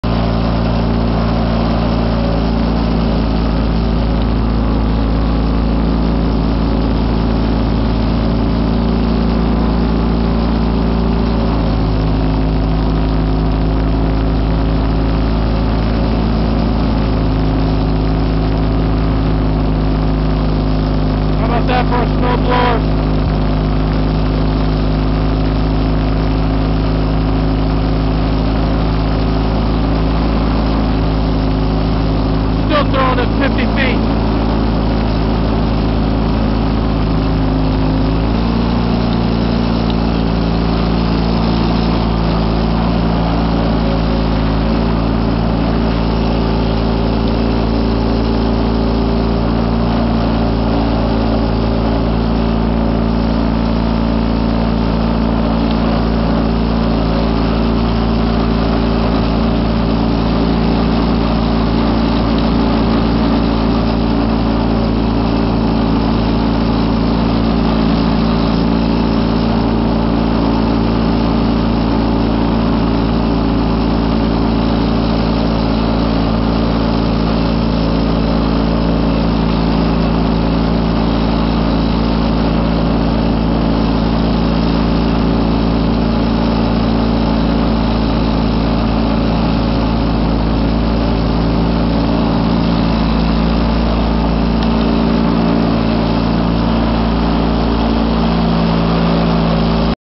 Wheel Horse C195 Snow Thrower Vs. Three Foot Snow Drifts
The evening of January 2nd 2014 left North East Ohio in 15 inches of snow and heavy drifting where I live.  Due to traffic going slow on my way home from work I had to clean out the driveway at night.  Still got an OK video of my snow thrower in a little action.  I rammed the unit into the snow drift and it collapsed.  It buried my unit and I just kept on going full 42 inch width and it still threw snow 40+ feet.  Cleaned out neighbors driveway which was one huge drift.  If the snow was wet engine would have worked pretty hard.  Wheel Horse snow thrower is Model No. 79360 Tall Shoot, 42 Inch Width, Single Stage only.  A C195 tractor weights in at 780 pounds; therefore, I have no extra ballast weight!  The original Kohler KT19 Series I (Horizontally Opposed 2 cylinder, 19HP) is still running strong with 2700+ hours on the meter.  The Series II KT17 & KT19 engines were pressurized lubricated engines.  Wish I had one of those!  Those of you with Wheel Horse tractors and want to run this model (tall shoot) snow blower need to get a tractor with the Kohler 14 HP or even the 16HP lung motor will have no worries!  I just like having the boxer motor style as every stroke is a power stroke!  This thing is almost as good as the Gravely MA210 Snow Cannon!